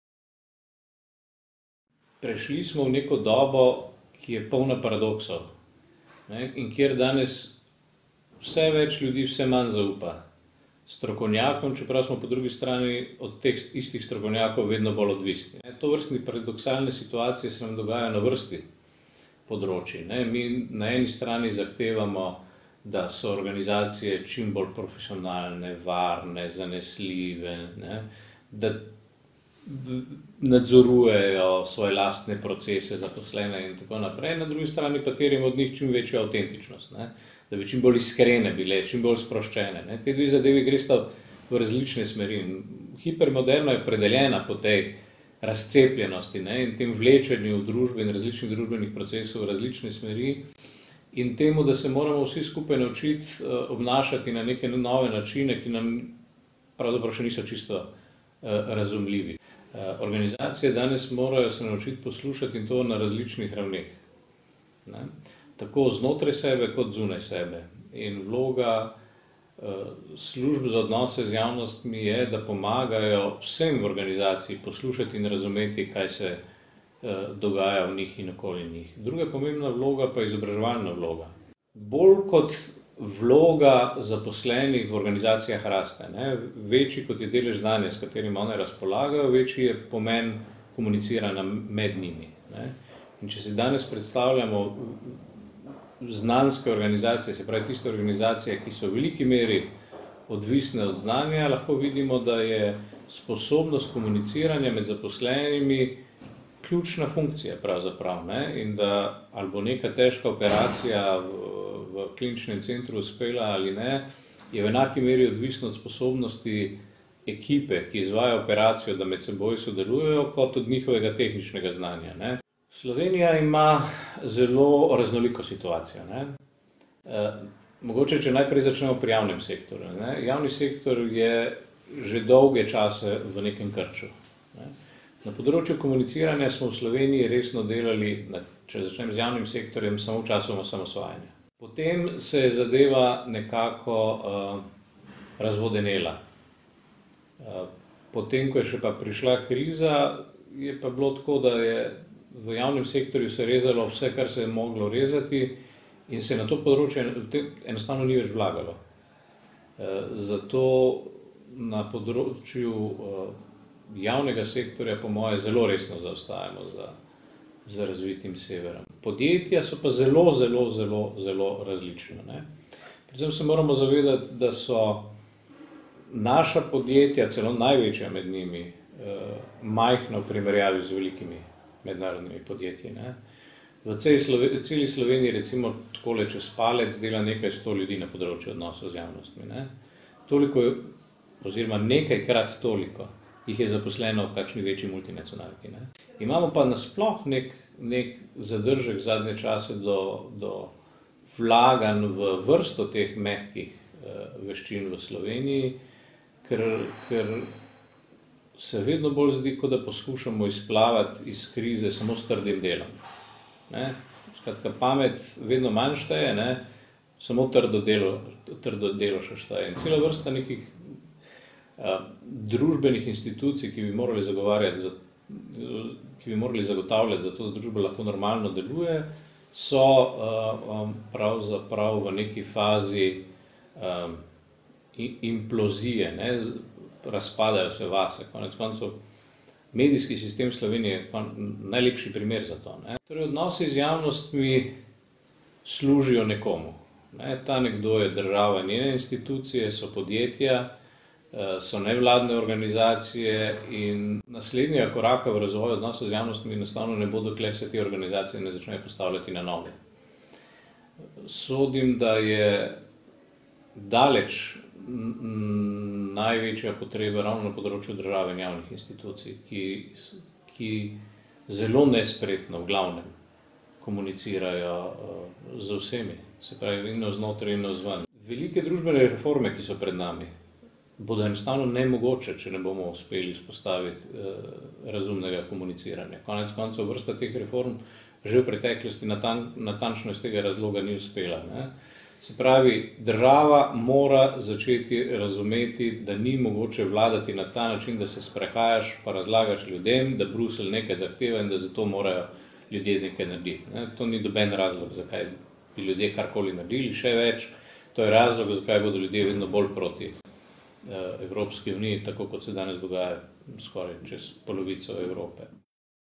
Dejan Verčič: Na ravni države in javnih institucij moramo vzpostaviti razumno komuniciranje
Pionir na področju odnosov z javnostmi v Sloveniji in profesor na ljubljanski fakulteti za družbene vede Dejan Verčič opaža, da država in javne institucije v Sloveniji zelo nespretno komunicirajo navznoter in navzven. Velike družbene reforme, ki so pred nami, bodo tako po njegovem mnenju nemogoče, če ne bomo uspeli vzpostaviti razumnega komuniciranja.